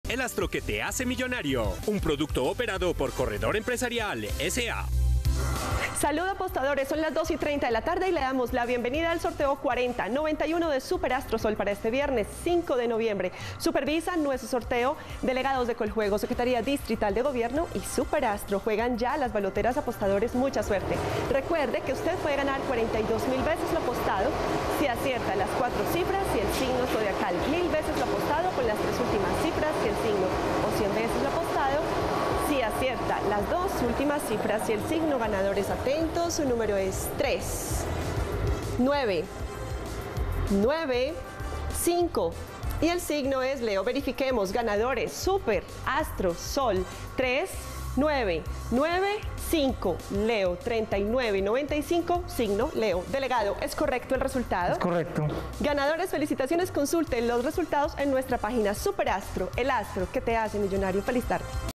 ASTRO SOL Resultado ASTRO SOL del viernes 05 de noviembre de 2021
Hola buenas tardes, te presentamos los resultados de las loterías y chances en Colombia, vamos con el Resultados del Chance SUPER ASTRO SOL del viernes 05 de noviembre de 2021.

Resultados de astro

Resultado astro SOL 05 de noviembre
Resultado LUNA 05 de noviembre
Resultado ASTRO SOL VIERNES 05 de noviembre